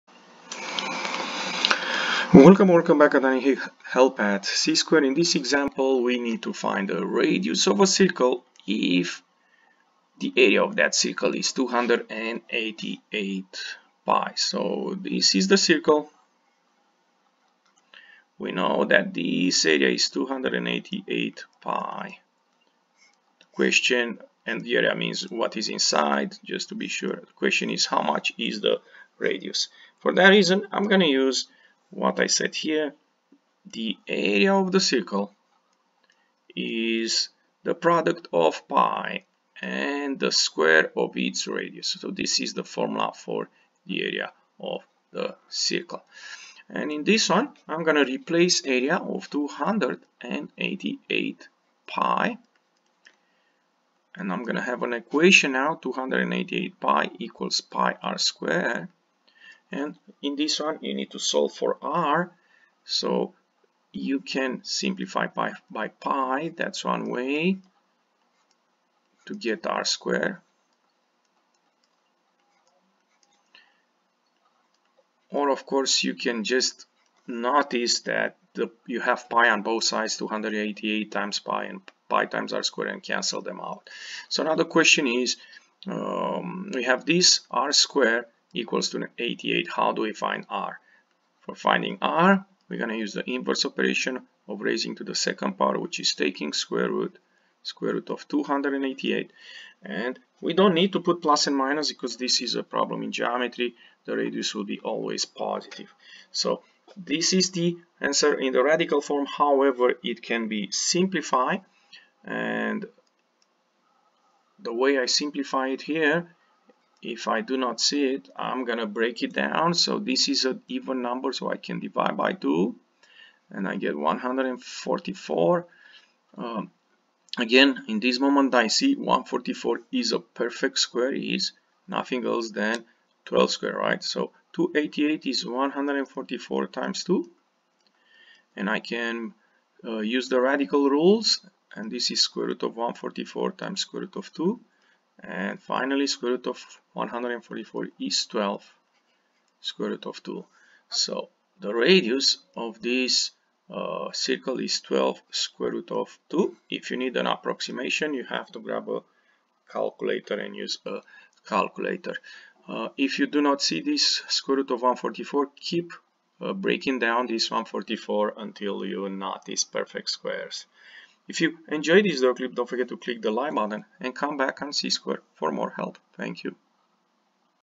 INH: Circle: Area of a Circle #2
This is an example of find the radius of a circle when the area of the circle is given.